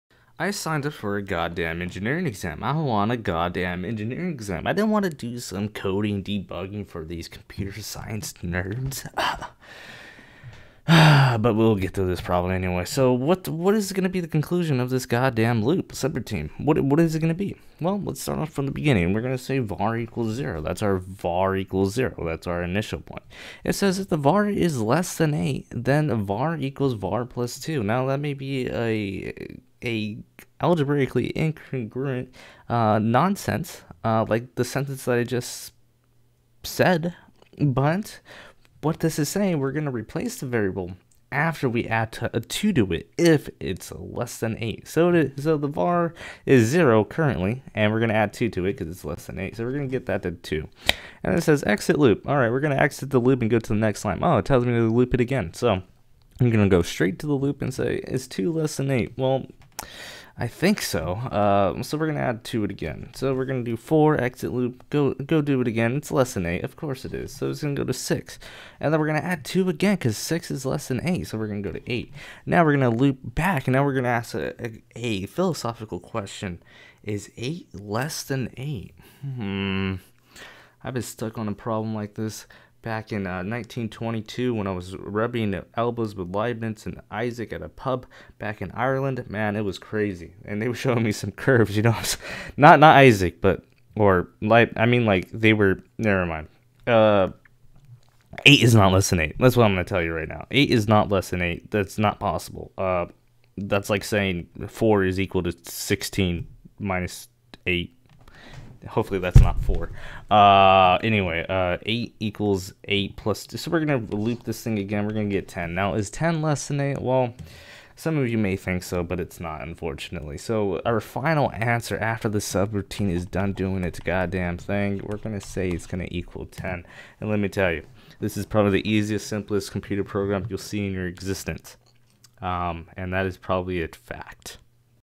FE Mechanical Exam Practice Problem #6: "The flowchart for a computer program contains the..."
FE Mechanical Exam Practice Problem #6:
"The flowchart for a computer program contains the following segment:

VAR = 0
IF VAR LESS THAN 8 THEN VAR = VAR + 2
ELSE EXIT LOOP
LOOP

What is the value of VAR at the conclusion of this routine?"

This problem test some basic knowledge of computer program execution lines logically.